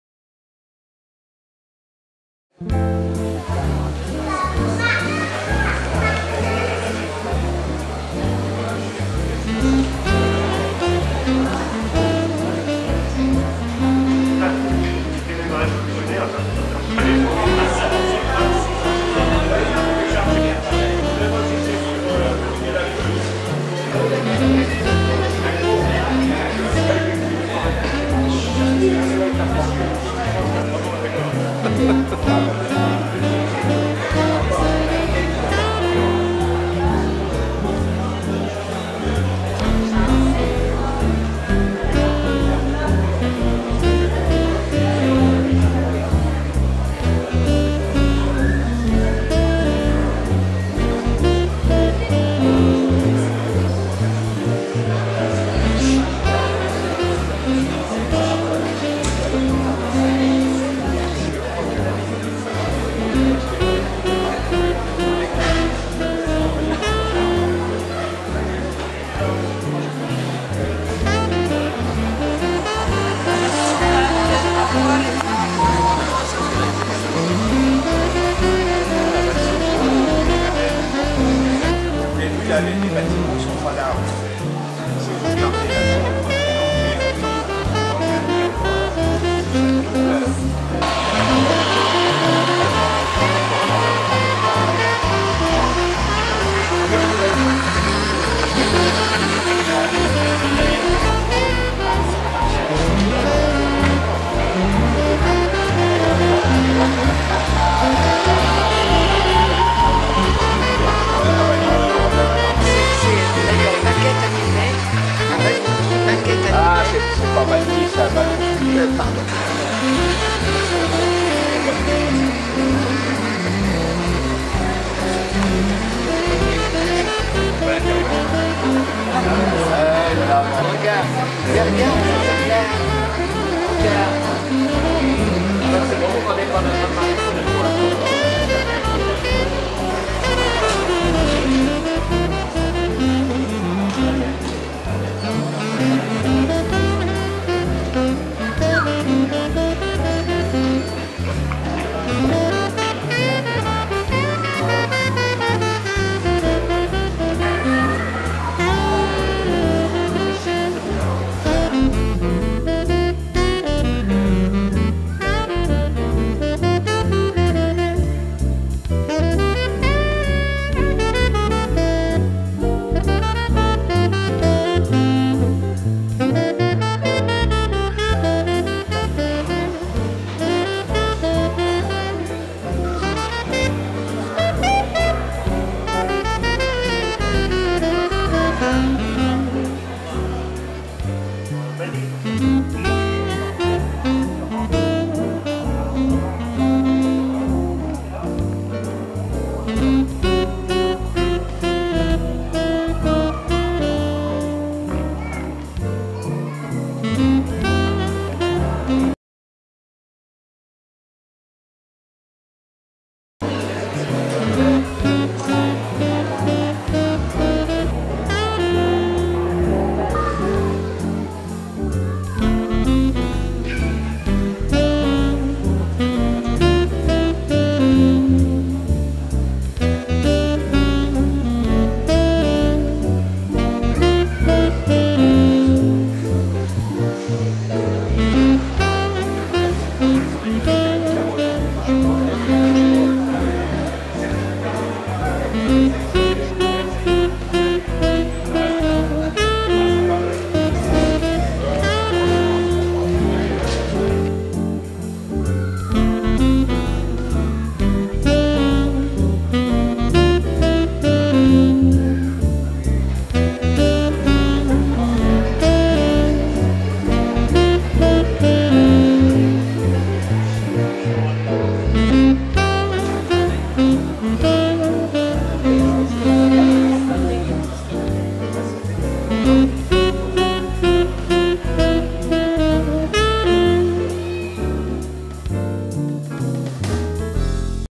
DOMPIERRE-SUR-BESBRE. (Allier). 3ème brocante de jouets et de miniatures (2014)
DOMPIERRE-SUR-BESBRE.  3ème brocante de jouets  et de miniatures  organisée  par le Rétro Mobile Club Dompierrois.
30 novembre 2014.